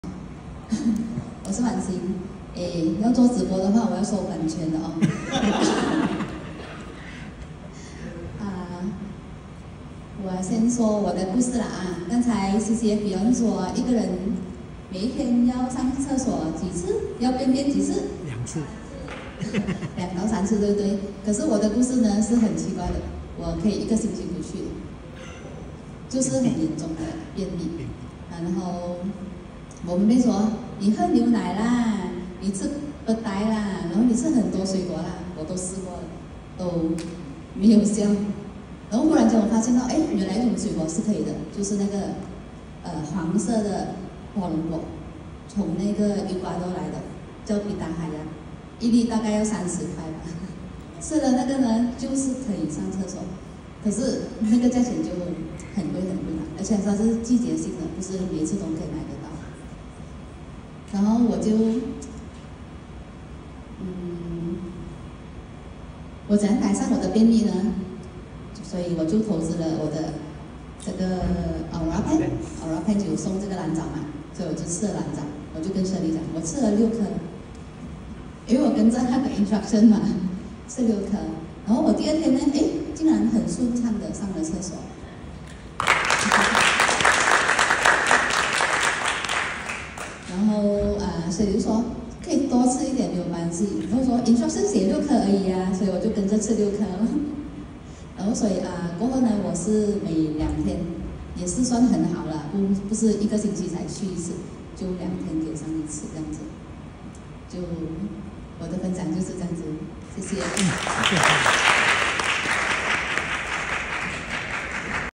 Belixz Spirulina testimony - Constipation
😱😱😱 啥？ 便秘 7天🤢
还要吃很贵的水果才能如厕😅
很高级的肠 😆

听她分享 BelixzSpirulina 蓝藻 怎么帮到她